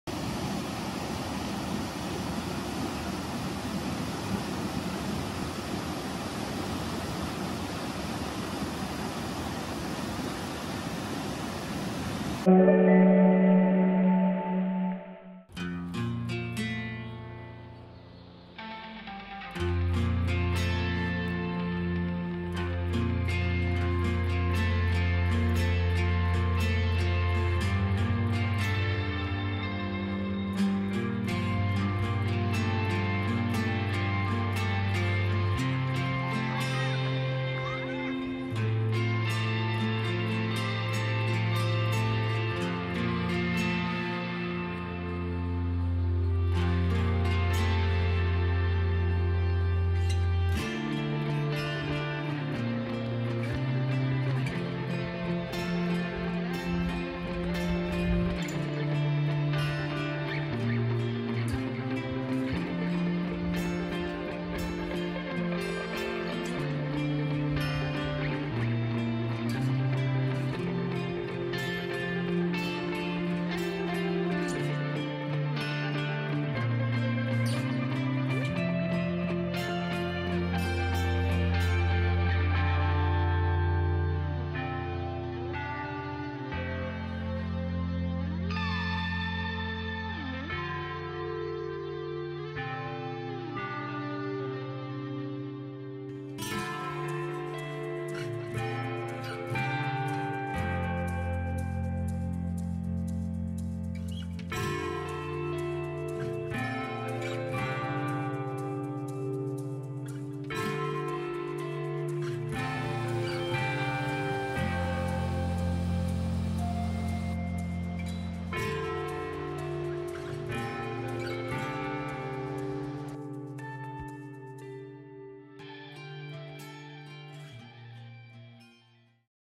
Cascate dell'Acqua Bianca, Alagna Valsesia (VC)
Partendo dal parcheggio del Wold, dopo Alagna Valsesia, dopo circa 40 minuti di sentiero si arriva all'Alpe Pile. Da qui si possono ammirare le cascate dell'Acqua Bianca.
Video girato con DJI Mini 2 ed editato con iPhone 13 mini